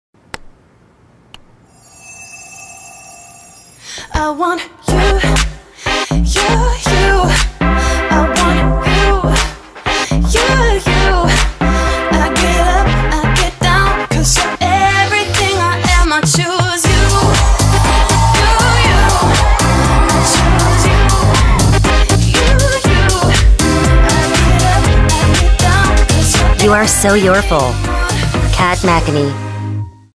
캣매코니(KAT MACONIE) '옹성우 뷰티 광고(설렘 주의)'
Colorist. Gitae Kim, Soomin Park

* 이 게시물은 CIC에서 후반 작업(DI)을 맡아 제작한 영상입니다.